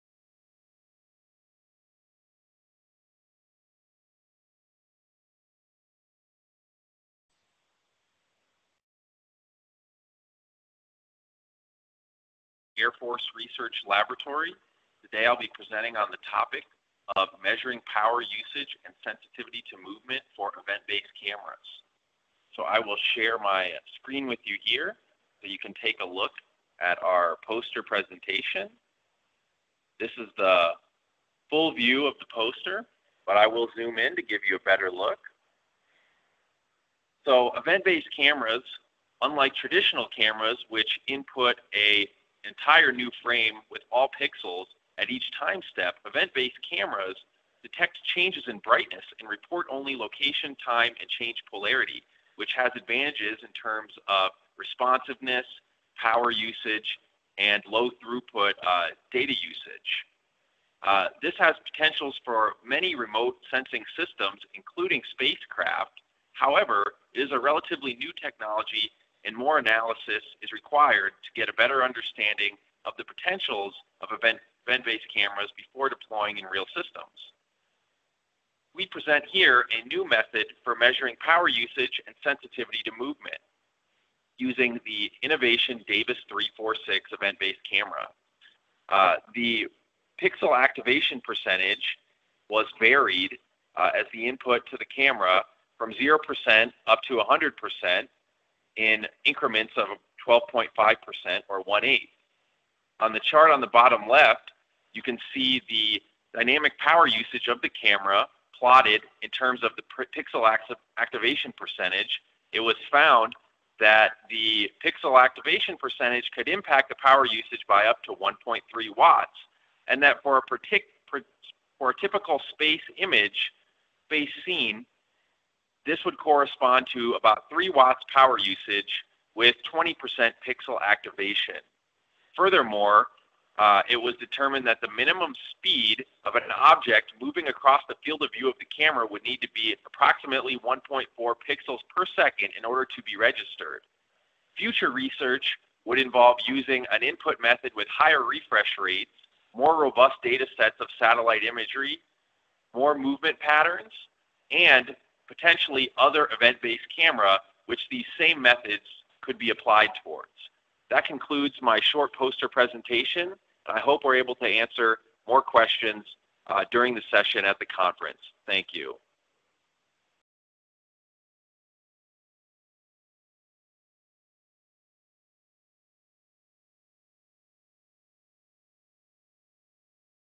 Measuring Power Usage and Sensitivity to Movement for Event-Based Cameras
Title: Measuring Power Usage and Sensitivity to Movement for Event-Based Cameras

Author: Christopher Voelkel{1}, Tyler Lovelly{2}, Andrew Pineda{2}, Peter McMahon-Crabtree{2}, Gabriel Mounce{2}

Abstract: Event-based cameras are emerging as potential new solutions for a variety of sensing applications. However, their capabilities must be better characterized before deploying in real systems. This research presents a new methodology to measure power usage and sensitivity to movement for event-based cameras, demonstrated with the iniVation DAVIS346. Power is measured with strobing images ranging from 0% to 100% pixel activation and with real-time satellite imagery. Results demonstrate that power usage varies by up to ~1.3W depending on pixel activation. A space-based system is estimated to use ~3W with ~20% pixel activation. Sensitivity is measured with an object moving across the field of view at varying speeds, and it was found that the object needed to move at a minimum of ~1.44 pixels per second to be registered by the camera. These methods can be applied to other event-based cameras and results can inform decisions about how to best deploy in real systems.